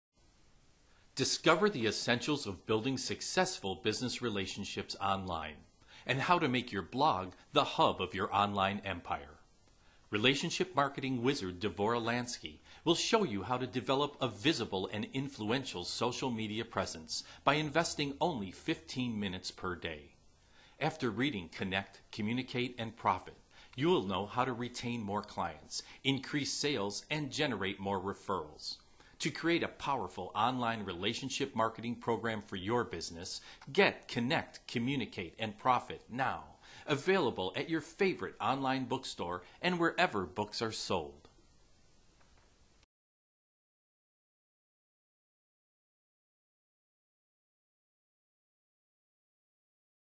Connect Communicate & Profit Book Video Trailer
Build your business by learning how to Connect, Communicate & Profit using the latest communication tools including social networking and blogs. Author marketing wizard D'vorah Lansky. Produced by authorsbroadcast.com